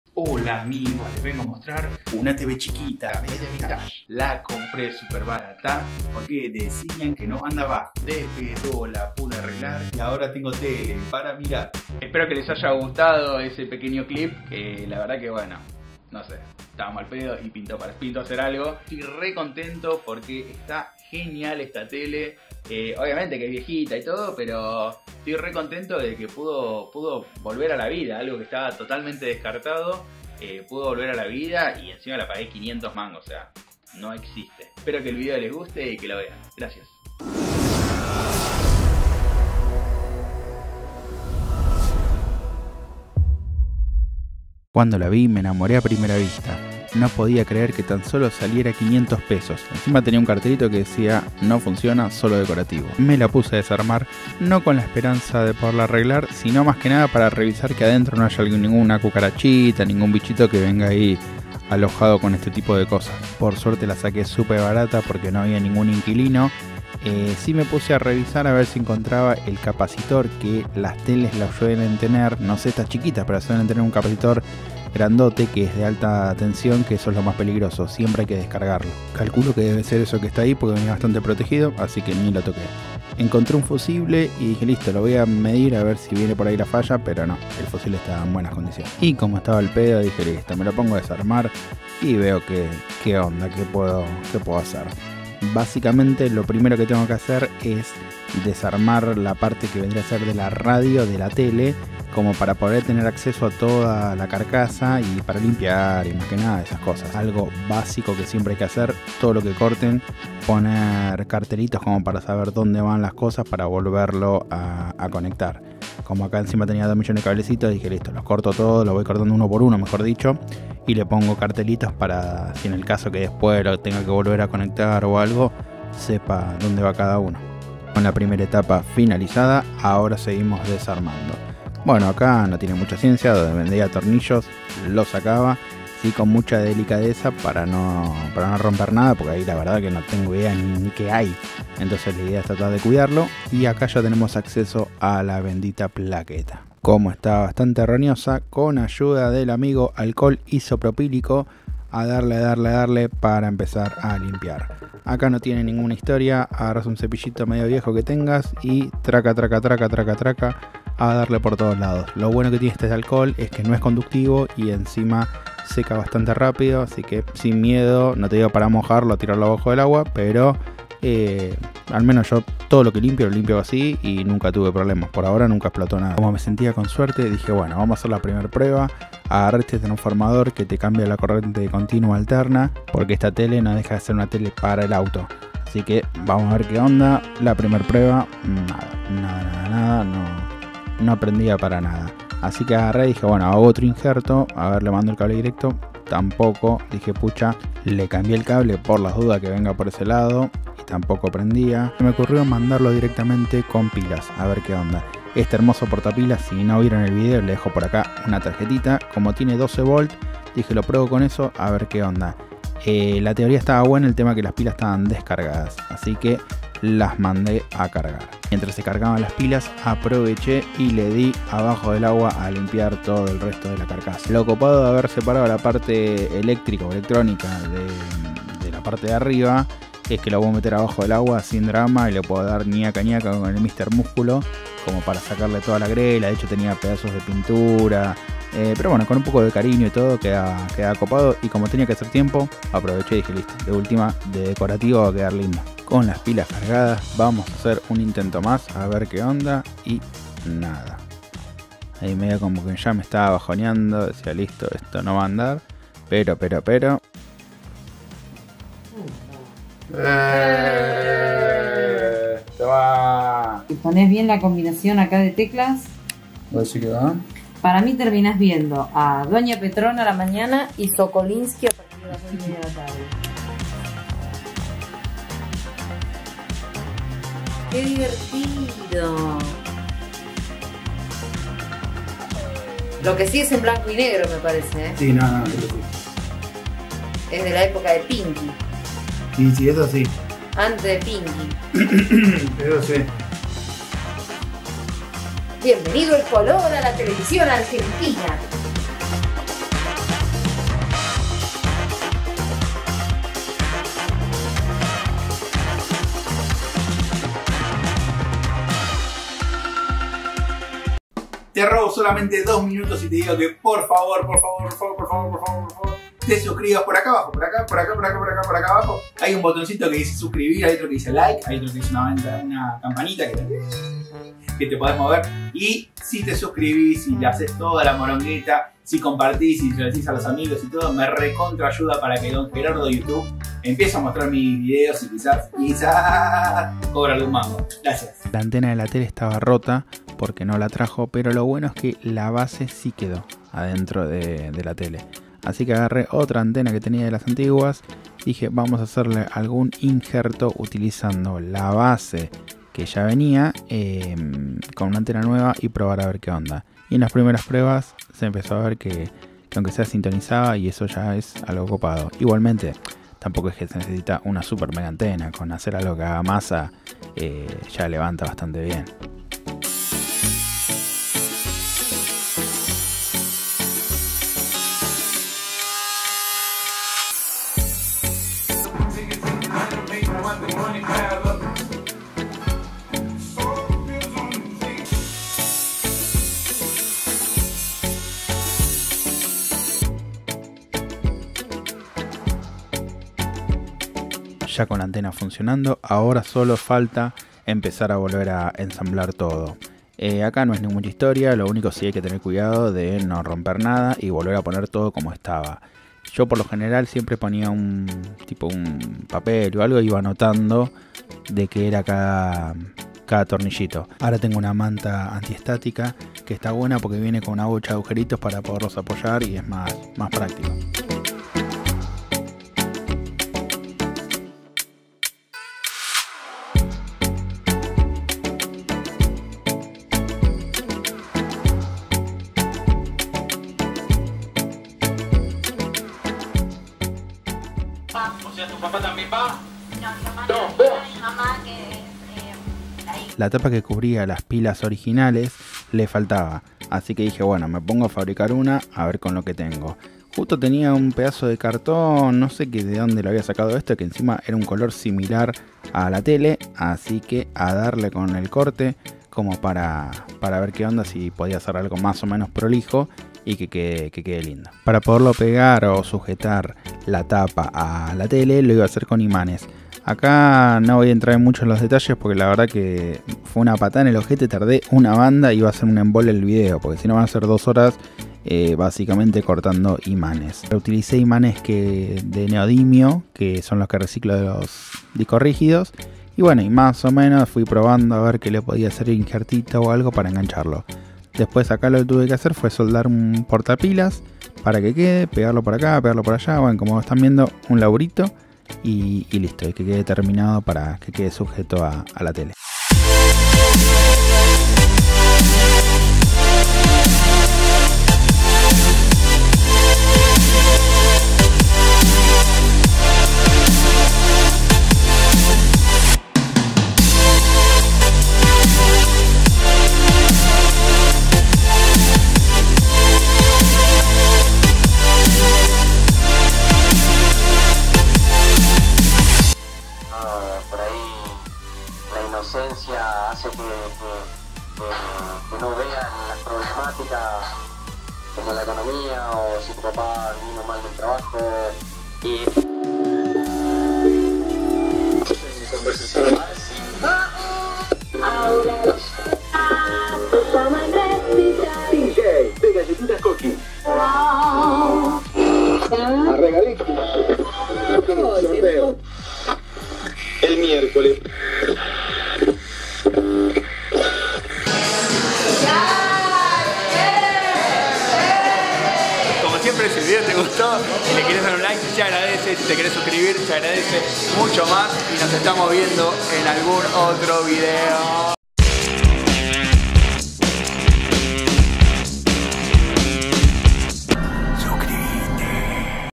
Como RESTAURAR una TV PORTATIL con RADIO AM/FM antigua Makrosonic DELUXE con muy POCO DINERO
Espero que el video les guste,

Este el VIDEO N 3 de toda UNA SEMANA de videos